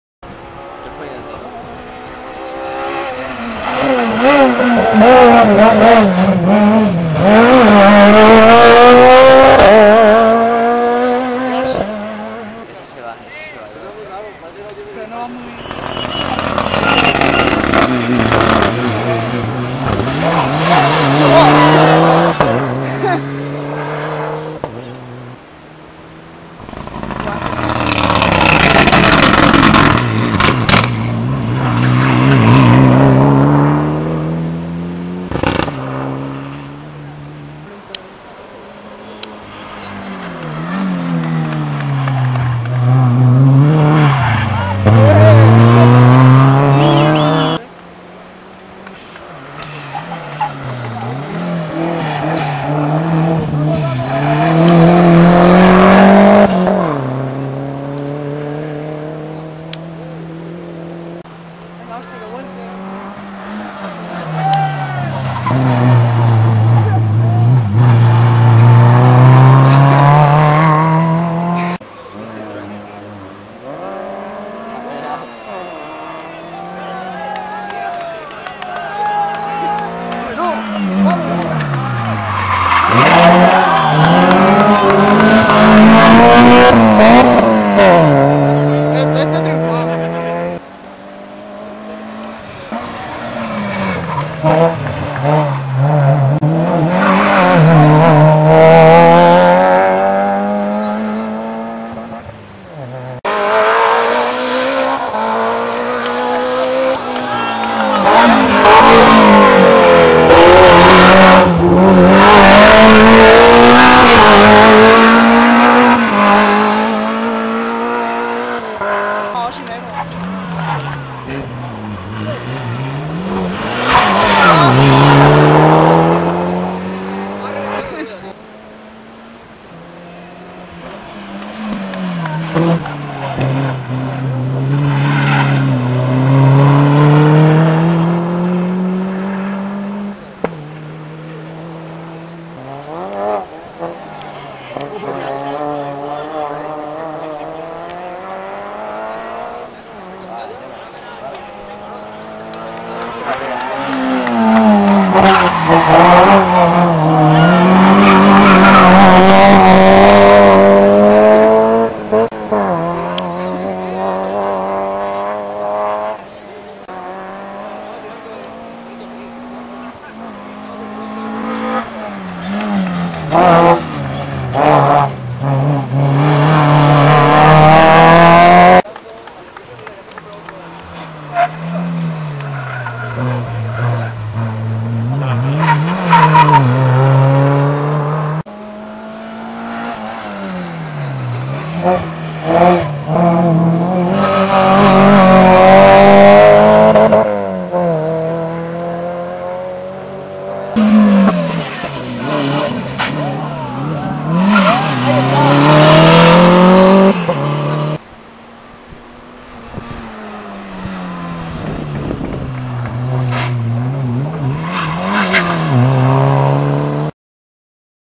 I Rallye Mutxamel
Tramo en Benifallim del I Rallye Mutxamel, 17 de Junio de 2007